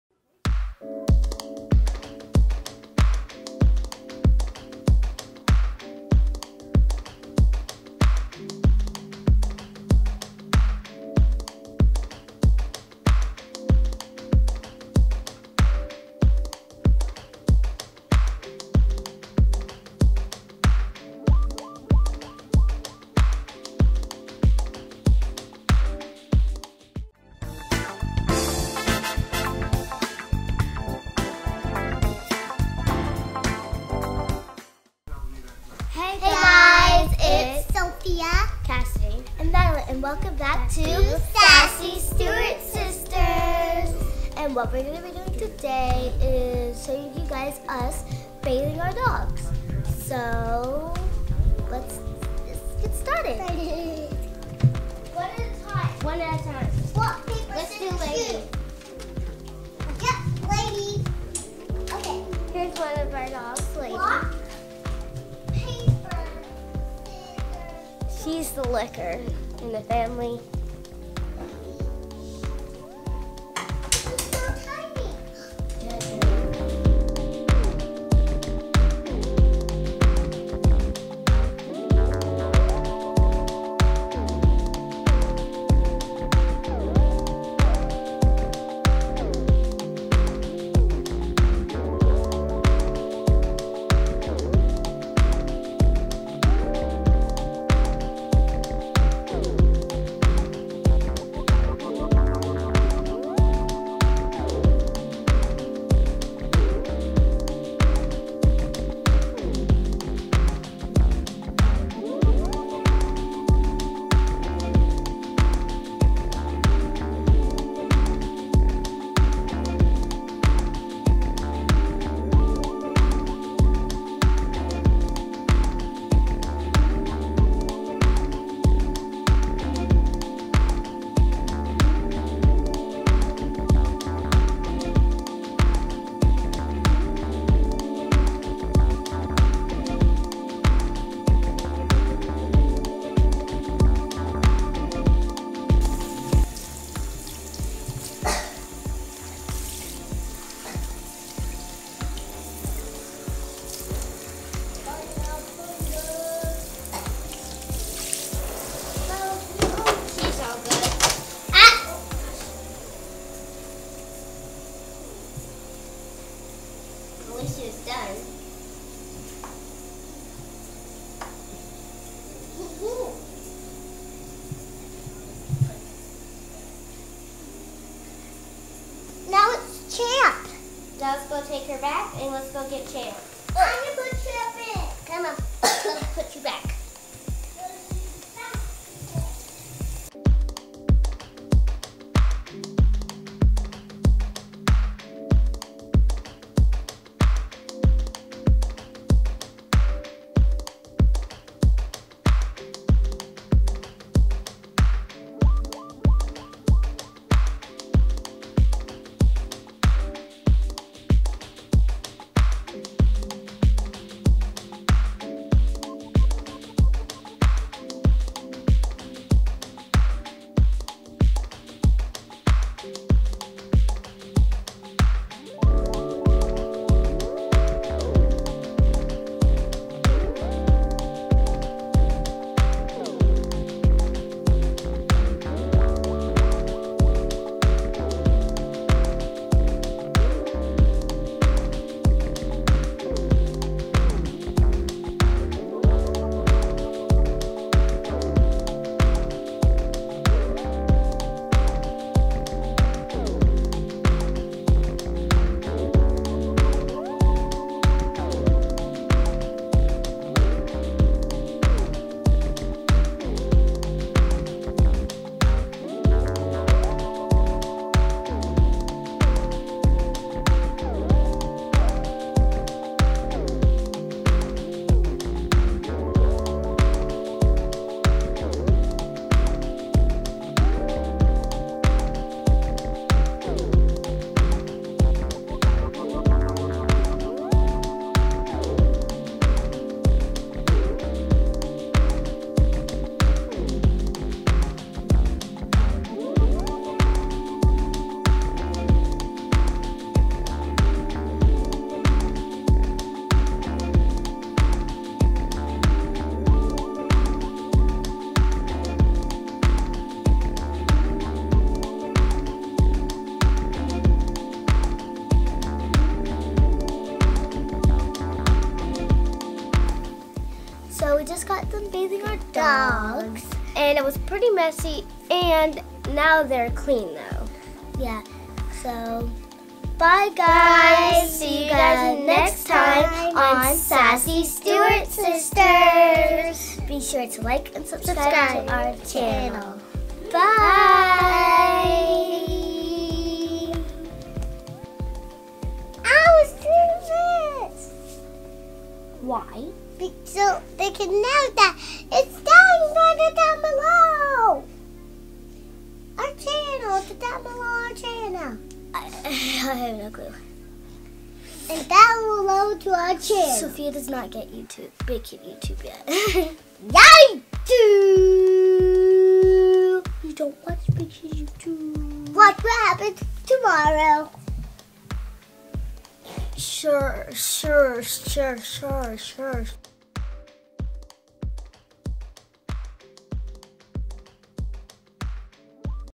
Washing the dogs
In this video we give our two aussie pups a bath. We are not the ones who usually give them a bath but we did in this video.